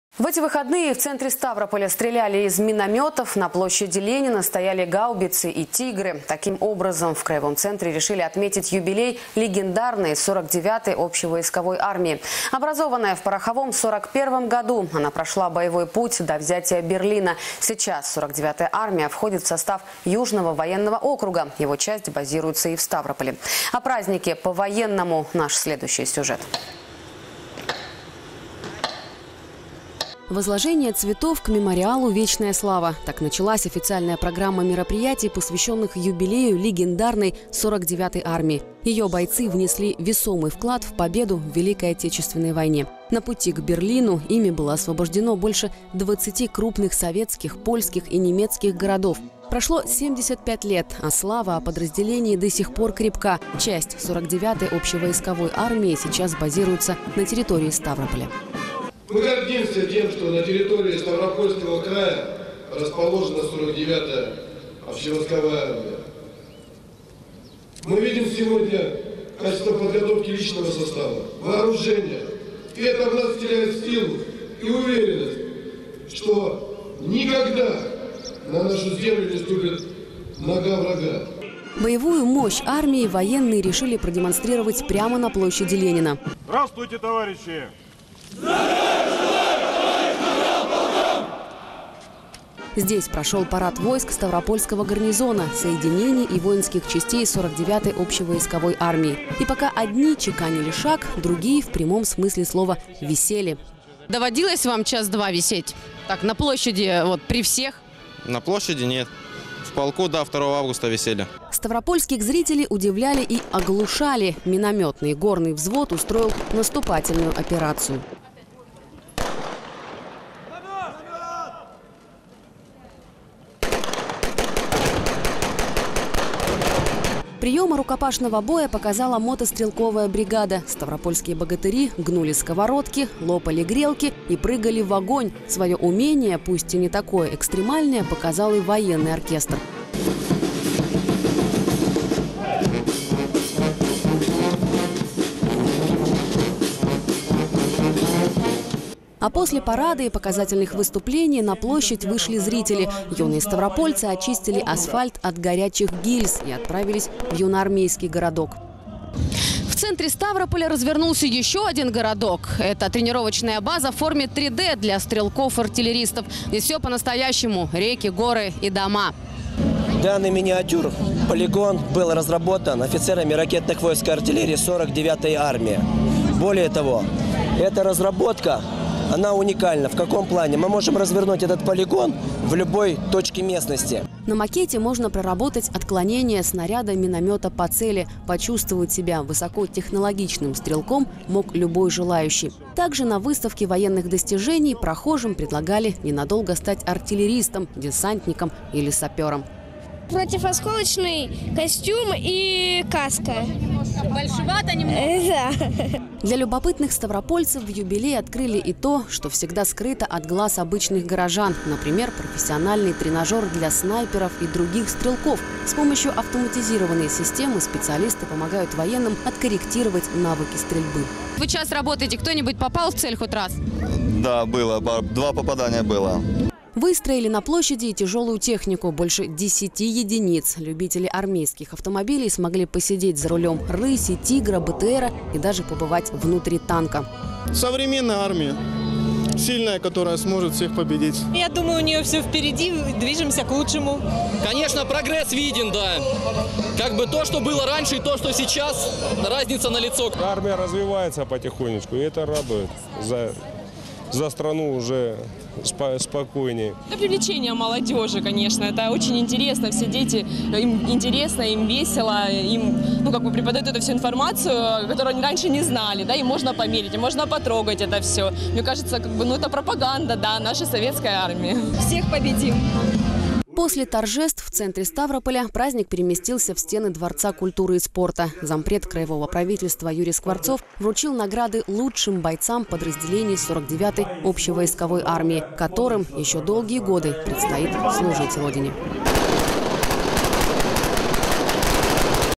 Легендарной 49 й армии 75 лет
В эти выходные в центре Ставрополя стреляли из миномётов, на площади Ленина стояли гаубицы и тигры. Таким образом в краевом центре решили отметить юбилей легендарной 49-й общевойсковой армии. Образованная в пороховом 41 году, она прошла боевой путь до взятия Берлина. Сейчас 49-я армия входит в состав Южного военного округа, его часть базируется и в Ставрополе.